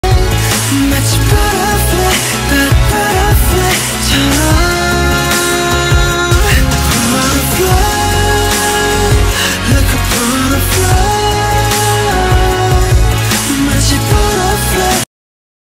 When you catch those bourbon butterflies 🦋🦋
Home is where the butterflies are! 🏠🏠
Today marks 2.5 years since we moved to Seattle, Washington. When the @bluerunspirits Flight Series was announced I knew I had to get a hold of these bottles. While I only hit on one during the initial release, a kind community member helped me get the other. 👊👊
Cheers to feeling at HOME. Whether that is a physical place, being with the right people, eating comfort food, or sitting back and enjoying the perfect pour. 🦋🦋🥃🥃